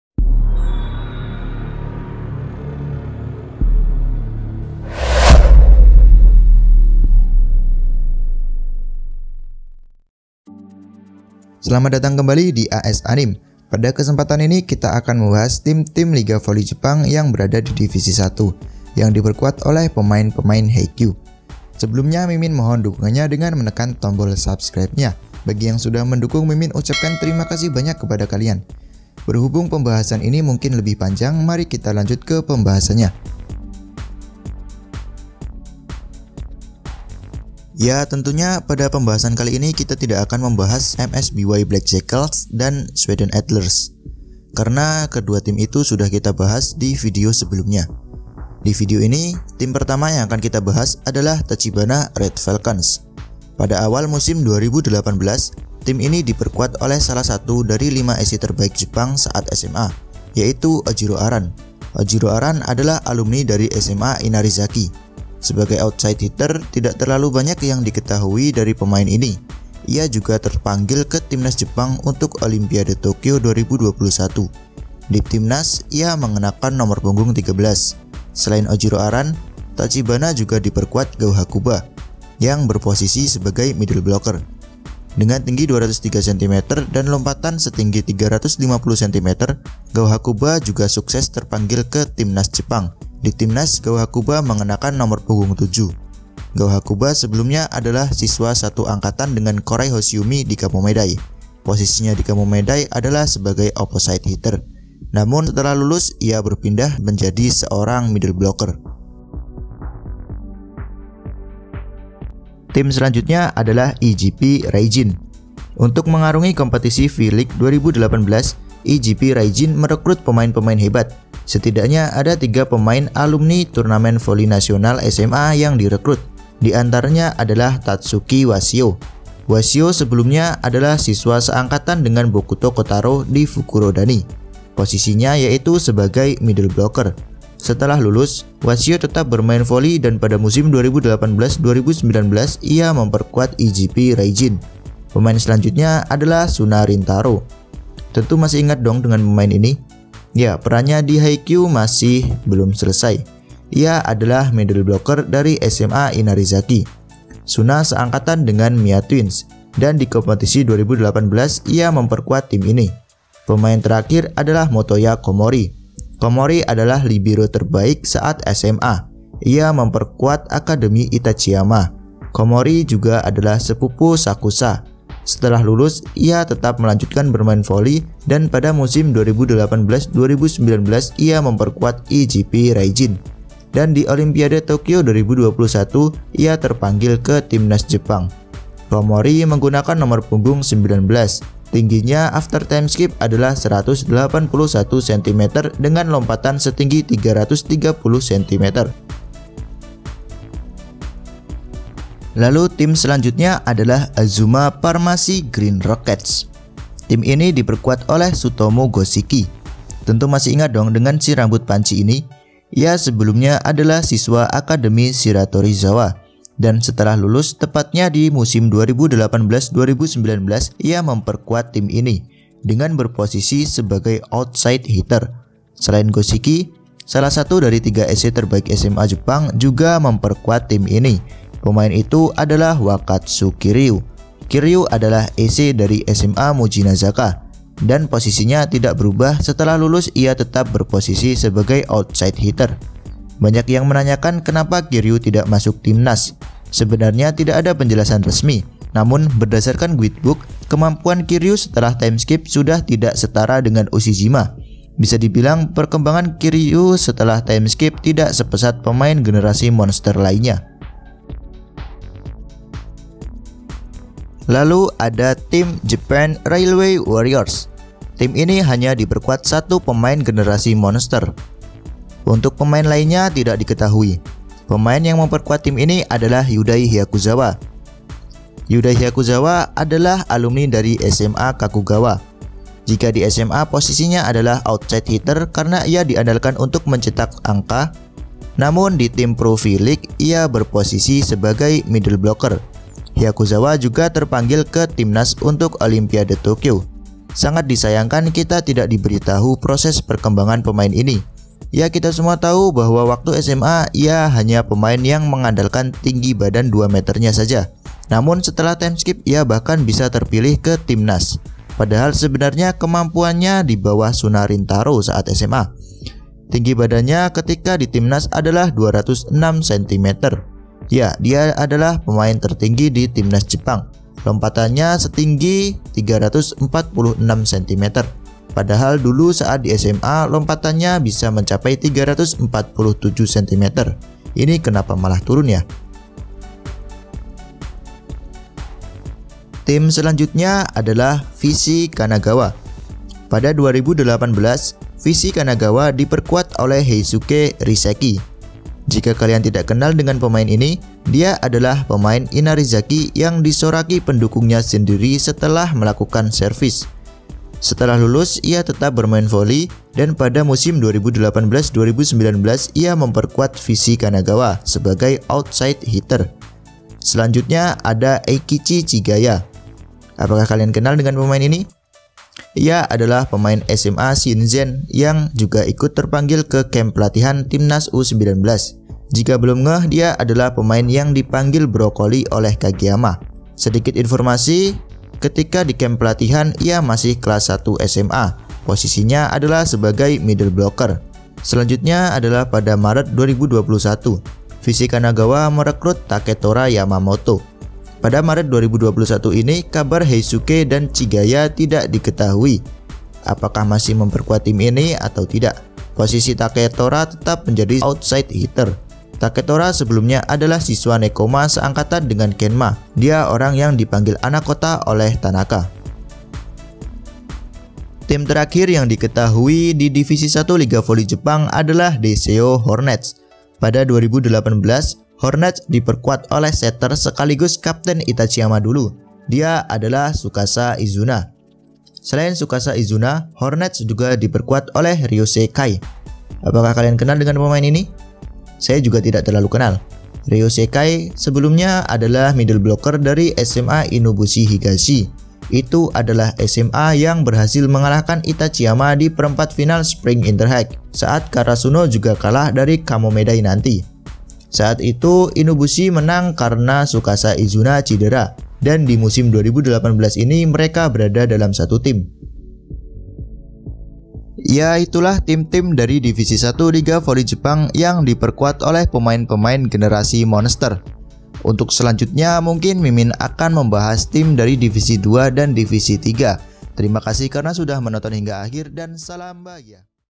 Tim liga voli jepang yang diperkuat bagian dari pemain generasi monster hakyuu!!
Hai teman teman kali ini kita akan membahas tim di divisi 1 liga voli jepang (v-league) yang diperkuat bagian dari pemain generasi monster . Di video ini saya tidak membahas MSBY BLACK JACKALS & SCHWEIDEN ADLERS karena 2 tim itu sudah kita bahas di video sebelumnya. Saya juga mengharapkan kemurahan hati teman teman untuk menekan tombol subscreb nya. Bagi yang sudah subscreb saya ucapkan banyak trimakasih 😊. Untuk link MSBY BLACK JACKALS &SCHWEIDEN ADLERS ada dibawah ya...